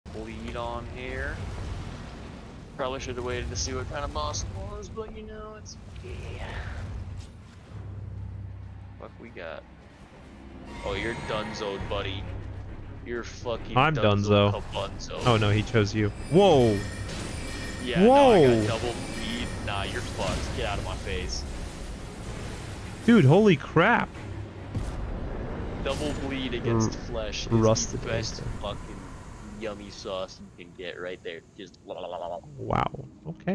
Didn't think you could slap a boss around so easily...
Amazing mod which allows you to experience multiplayer without as much pain and wasted time: Seamless Co-op